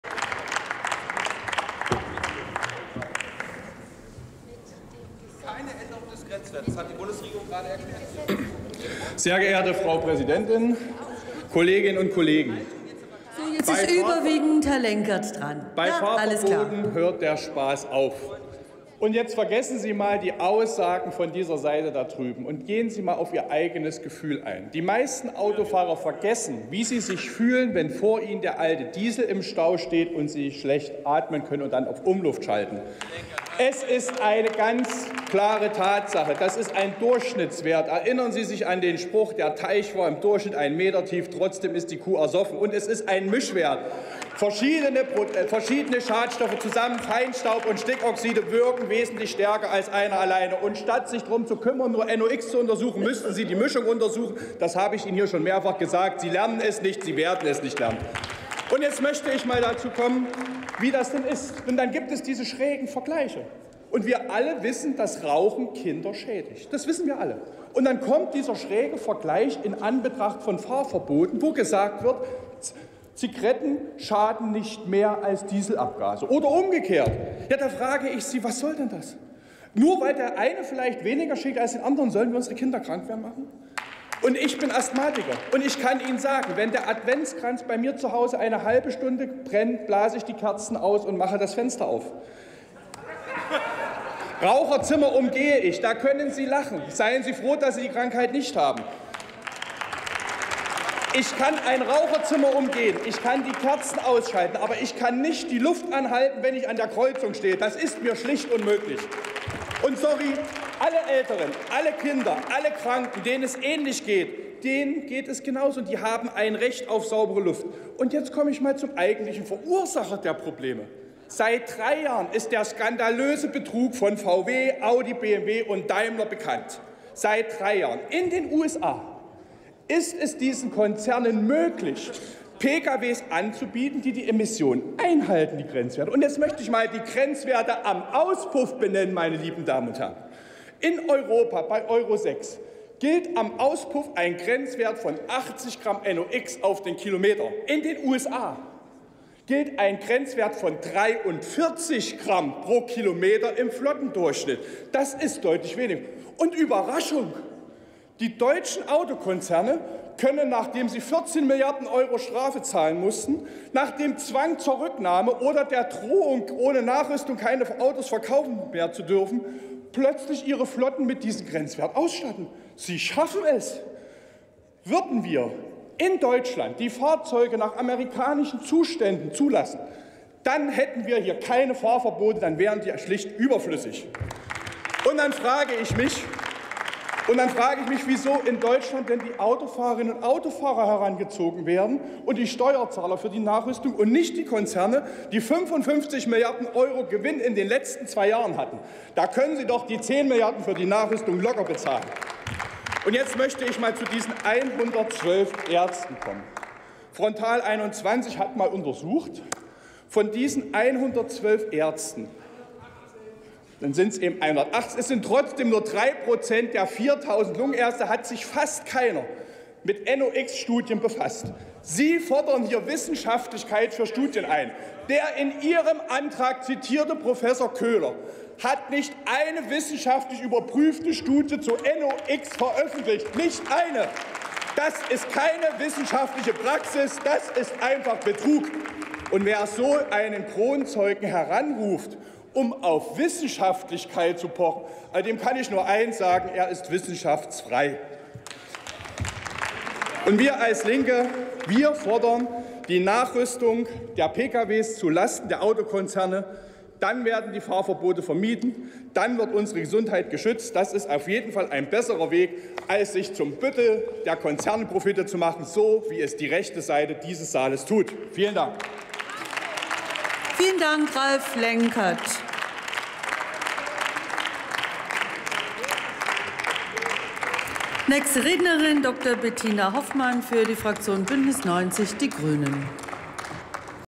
Ralph Lenkert, DIE LINKE: Fahrverbote verhindern, Diesel nachrüsten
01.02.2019 - DIE LINKE fordert: Die betrügerischen Autokonzerne VW, BMW, Daimler und Co. sollen die technische Nachrüstung von Diesel-Autos zahlen, und nicht die SteuerzahlerInnen oder AutofahrerInnen! Fahrverbote braucht es nicht, wenn Grenzwerte eingehalten werden. Für Kinder und Menschen mit Krankheiten ist dies dringend geboten.